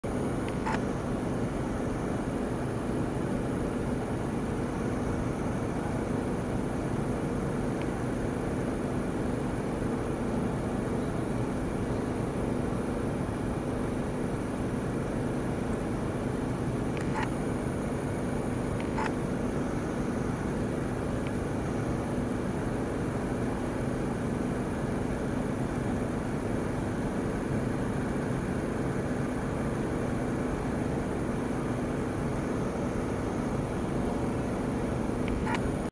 What is this cute bird I saw from my apartment in Ibaraki Japan? 🐤
These days I am not a passionate birder anymore, so it's a bit hard to identify this bird. What is this bird?